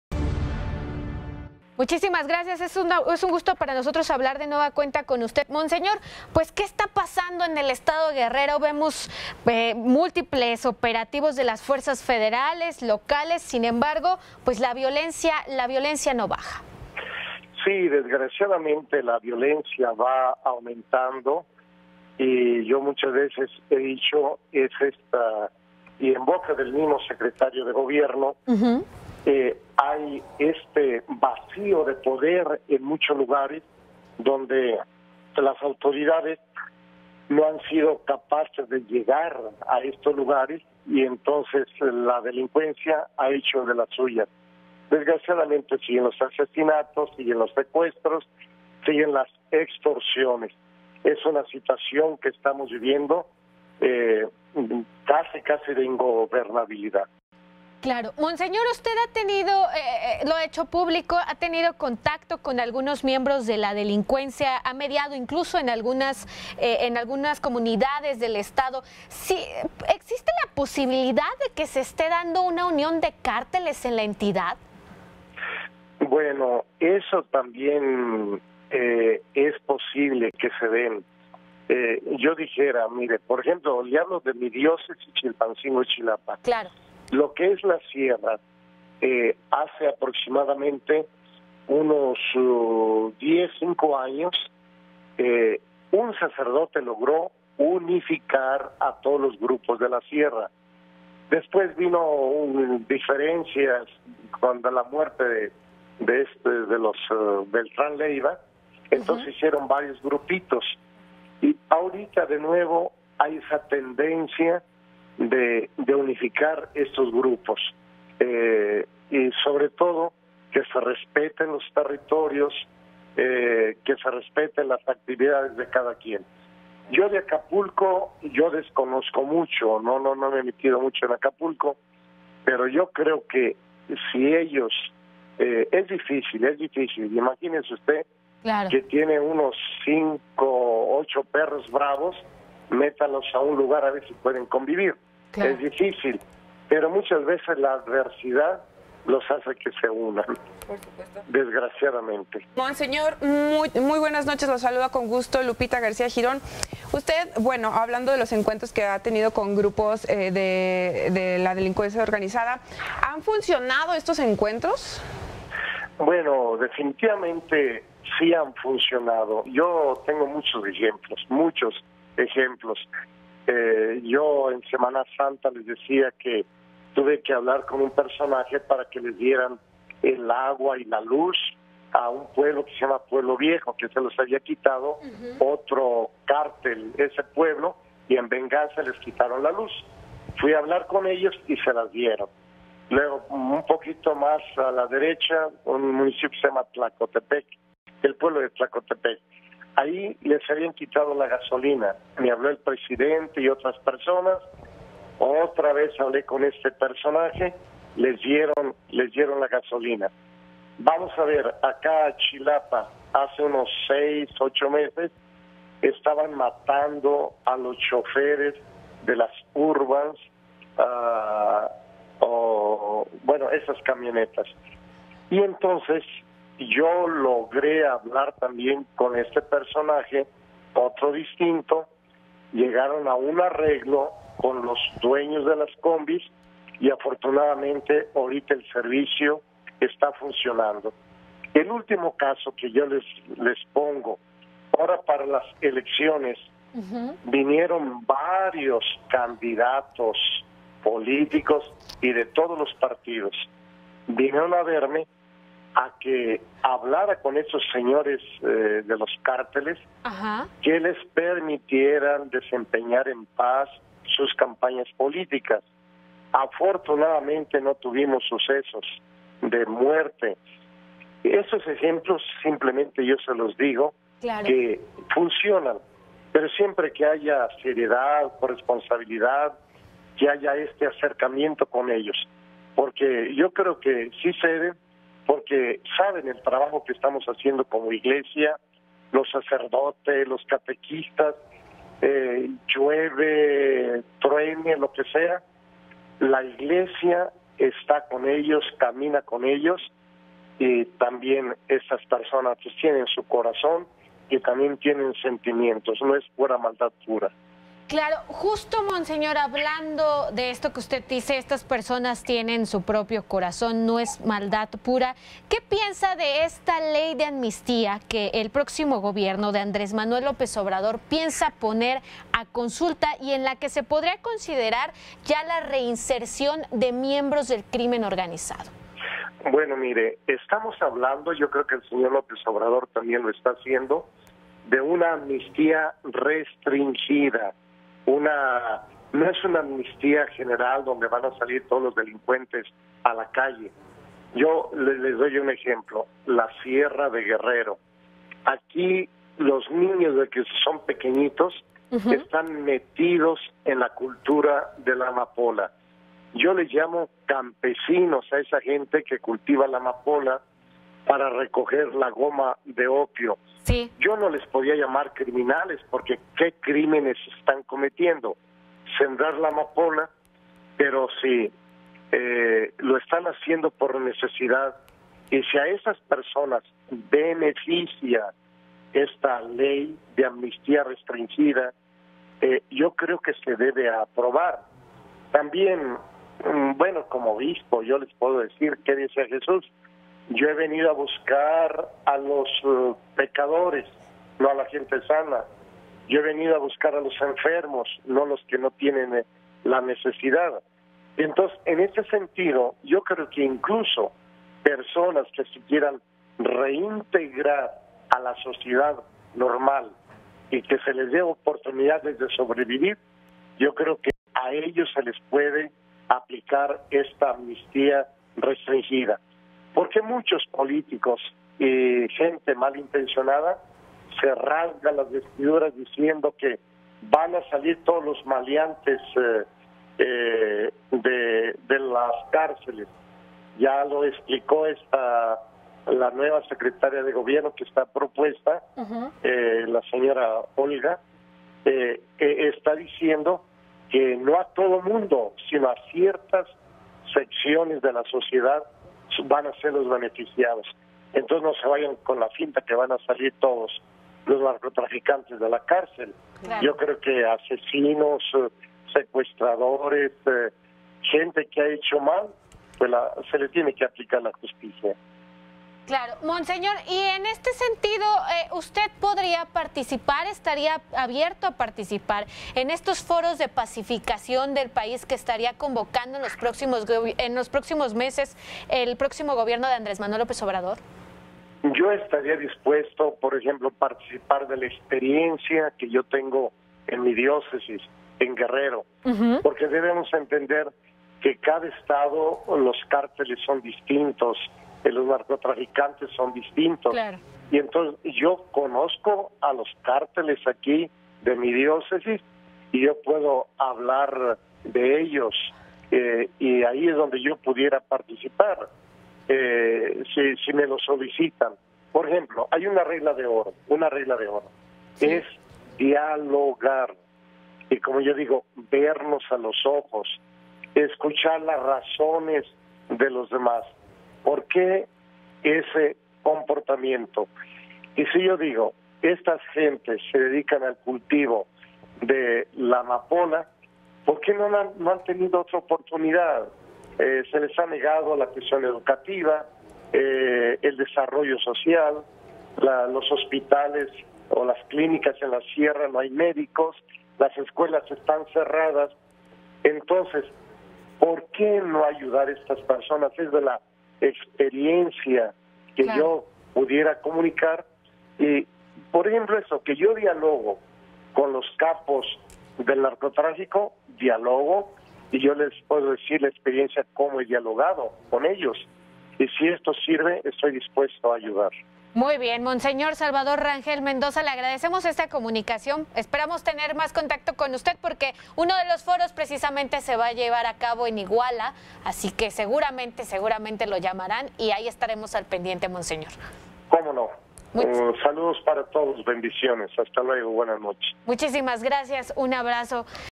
Intervención de la iglesia en la violencia en Guerrero / Salvador Rangel - Entrevista 23 de julio
¿Cómo ve la iglesia la propuesta de amnistía de AMLO? ¿La intervención de sacerdotes ante la violencia ayuda? Monseñor Salvador Rangel de Chilpancingo nos da su opinión